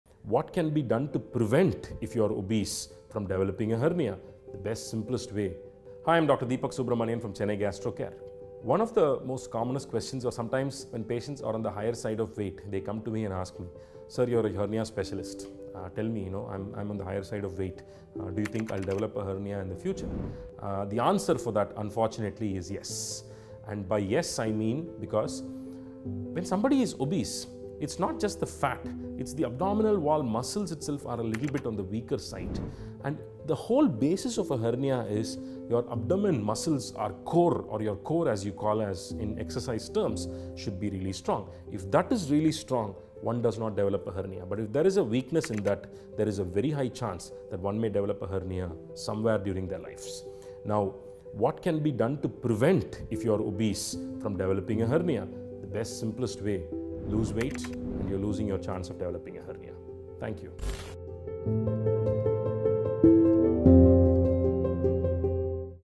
Does Obesity Increase The Risk of Hernia? | Dr. Deepak Subramanian
In this informative video, Dr. Deepak Subramanian discusses the link between obesity and hernia. He explains how excess weight puts additional pressure on the abdominal muscles, which can weaken them and cause a hernia to develop. Dr. Subramanian also outlines the various types of hernias and their symptoms and emphasizes the importance of seeking medical attention if one suspects they may have a hernia. He also provides practical tips on how to reduce the risk of hernia, including maintaining a healthy weight through proper diet and exercise.